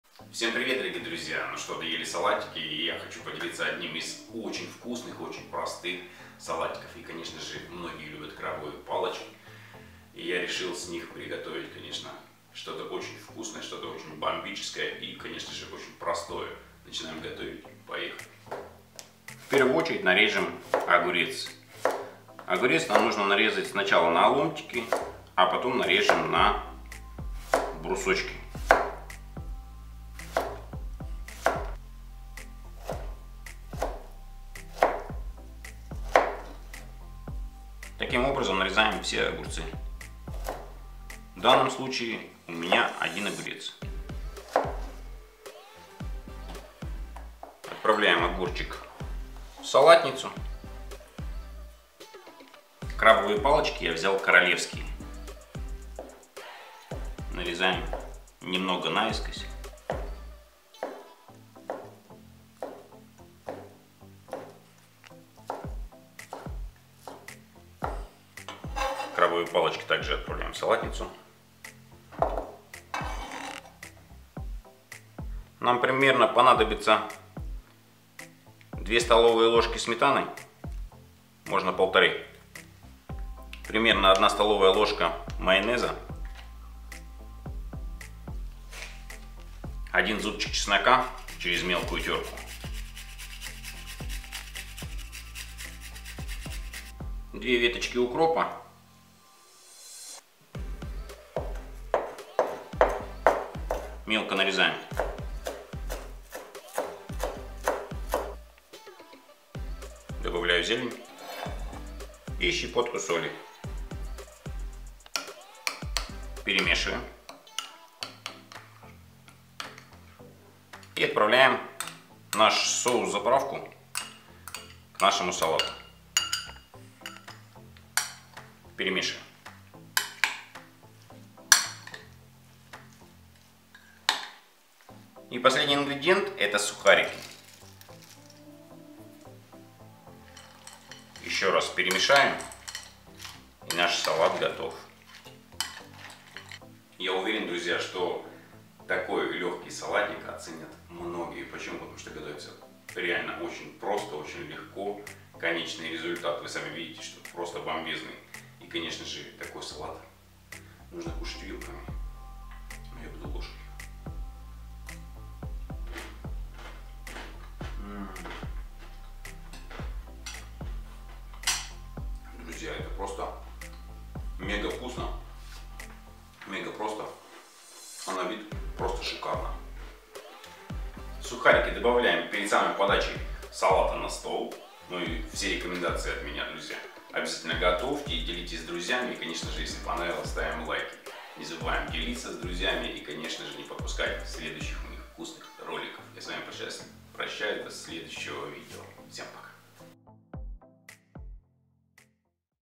САЛАТ КРАБОВЫЙ! На самом деле все просто и НЕВЕРОЯТНО вкусно!
Готовлю два раза в день! Все в ШОКЕ от такой ВКУСНОТЫ!
Один из самых ВКУСНЫХ  салатов из крабовых палочек.
Приятного аппетита!

ИНГРЕДИЕНТЫ.
огурец 1шт
крабовые палочки 1 пачка (у меня королевские)
укроп
соль
сухарики